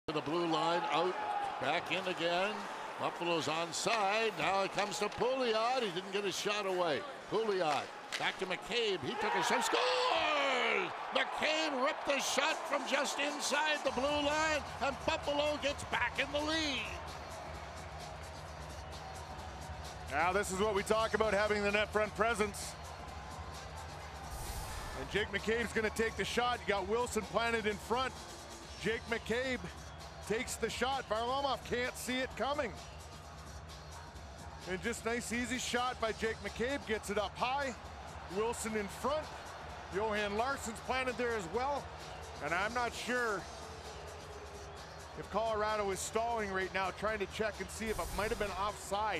Jake McCabe Goal vs COL 12-05-17
Jake McCabe 1 goal of 2017-18 nhl season. even goal assisted by Benoit Pouliot and Johan Larsson